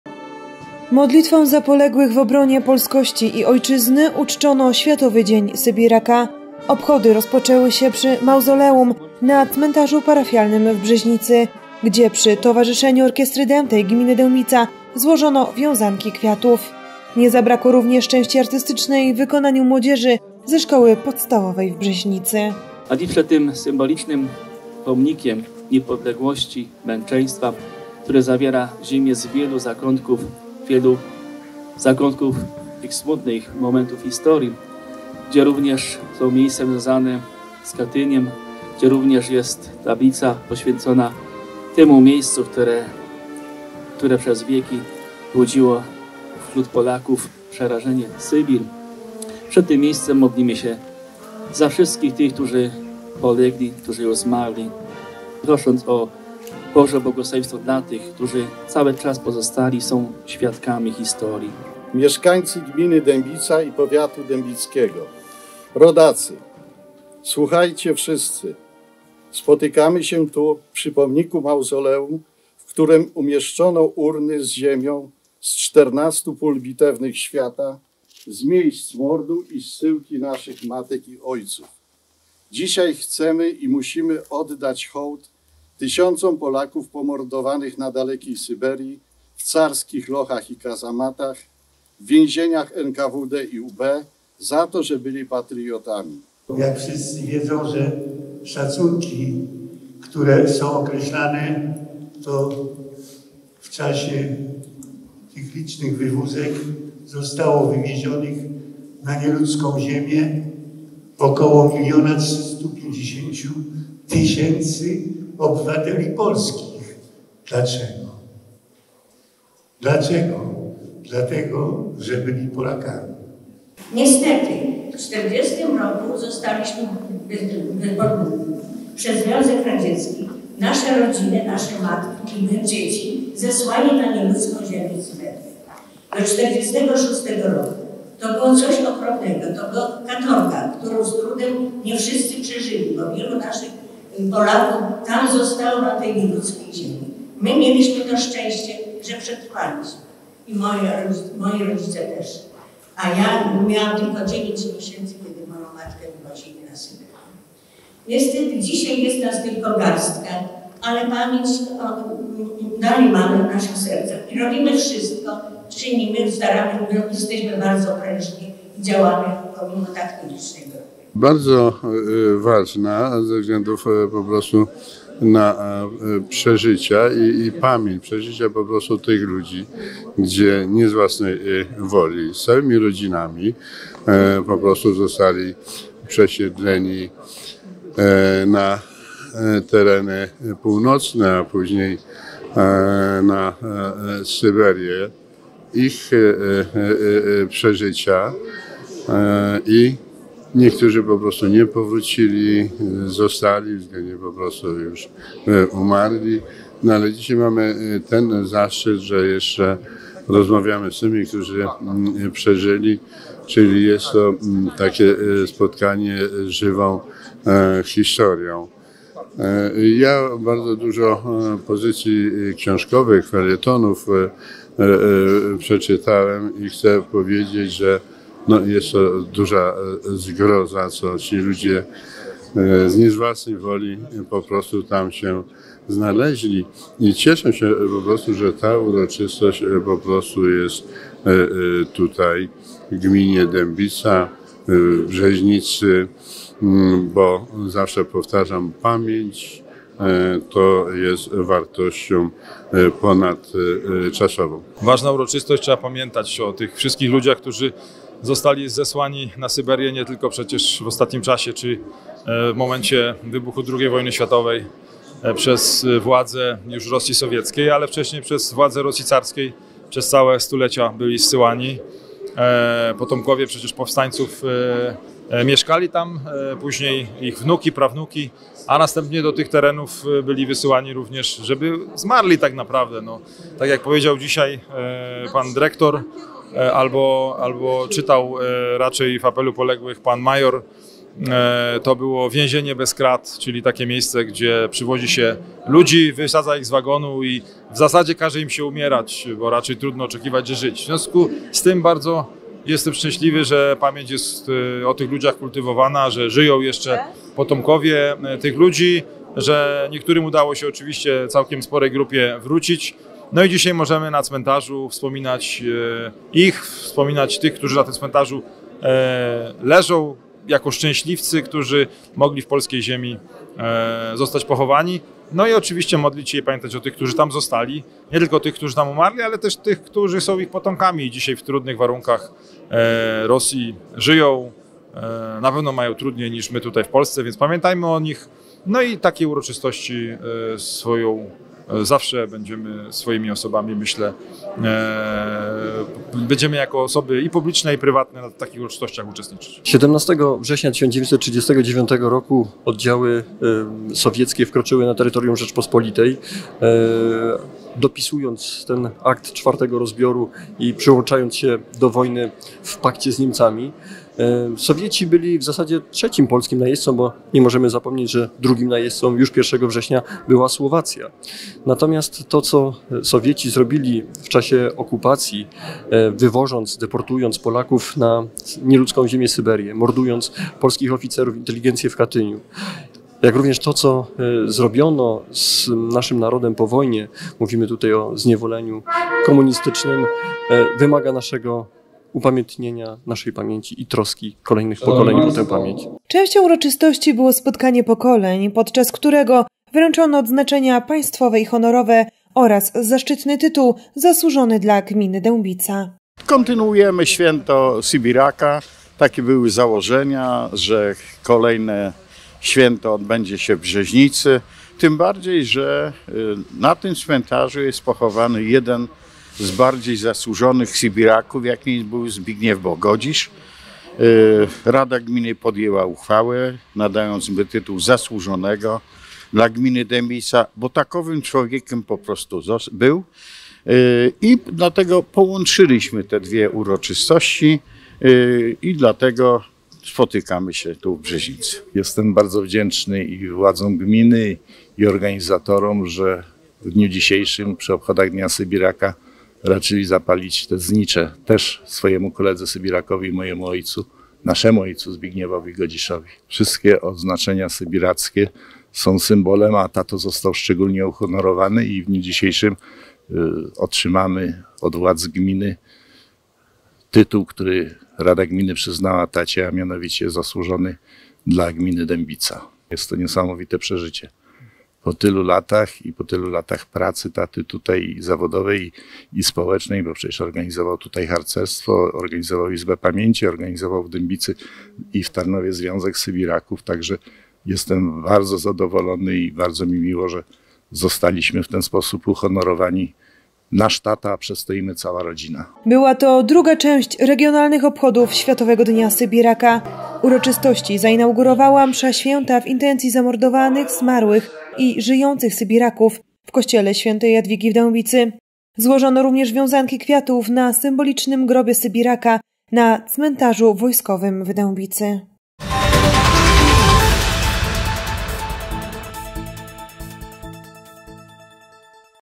Modlitwą za poległych w obronie Polskości i Ojczyzny uczczono Światowy Dzień Sybiraka
Modlitwą  za  poległych w obronie Polskości i Ojczyzny uczczono Światowy Dzień Sybiraka. Obchody rozpoczęły się przy Mauzoleum na  Cmentarzu  Parafialnym  w  Brzeźnicy, gdzie przy towarzyszeniu Orkiestry  Dętej - Gmina Dębica złożono wiązanki kwiatów.  Nie zabrakło również części artystycznej w wykonaniu młodzieży ze Szkoły Podstawowej w Brzeźnicy.
Częścią uroczystości było spotkanie pokoleń, podczas którego wręczono odznaczenia państwowe i honorowe oraz zaszczytny tytuł "Zasłużony  dla  Gminy Dębica".
Była to druga część regionalnych obchodów Światowego Dnia Sybiraka. Uroczystości zainaugurowała Msza  Św. w  intencji  zamordowanych,  zmarłych  i  żyjących  Sybiraków w Kościele  Św. Jadwigi  w  Dębicy. Złożono również wiązanki kwiatów  na  Symbolicznym  Grobie  Sybiraka na Cmentarzu  Wojskowym  w  Dębicy.